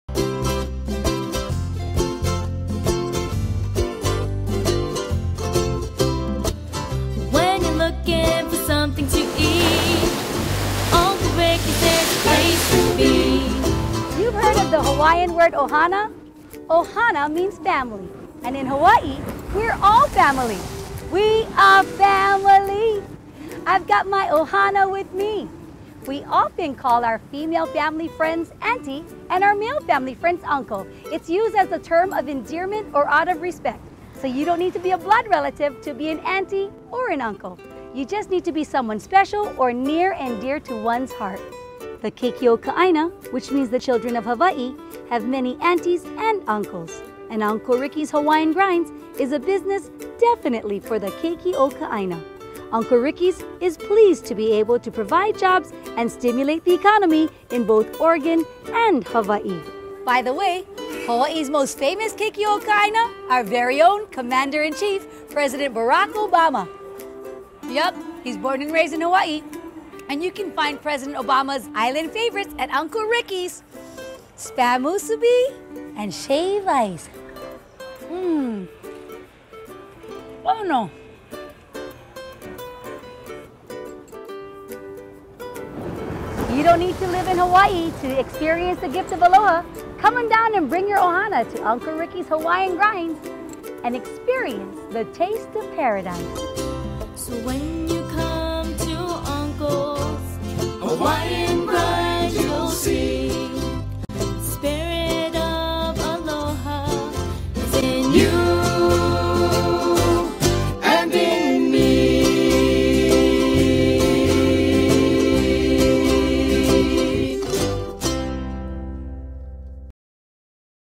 Uncle Ricky's Hawaiian Grinds - About Us
Uncle Ricky's Hawaiian Grinds in Corvalis, Oregon serves up great local hawaiian-style food and aloha.

today!